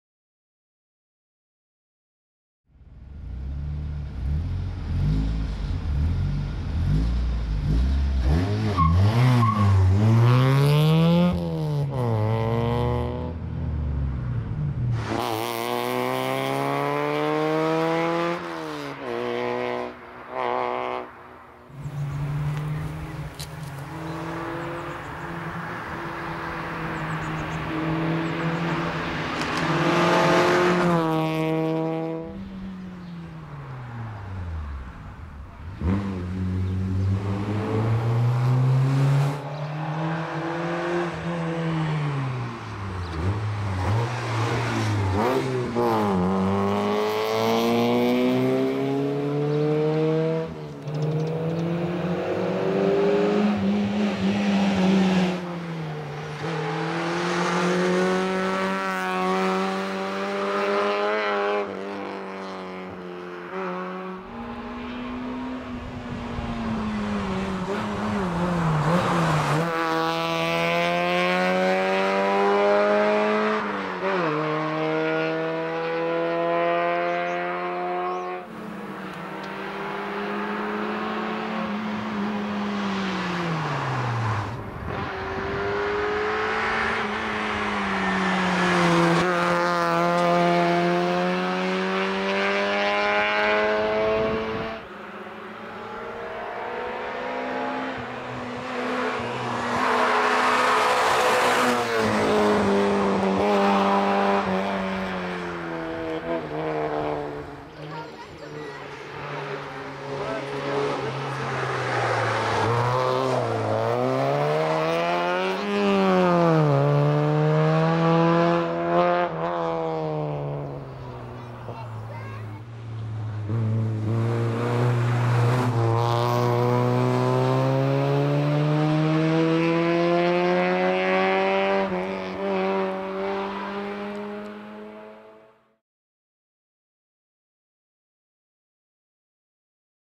Boženka Křížová - Citroën Saxo VTS - Makarov 2021 - EDDA CUP
Boženka Křížová si s vozem Citroën Saxo VTS dojela pro osmé místo ve třídě Saxo Cup v závodech do vrchu Makarov 2021 v seriálu EDDA CUP.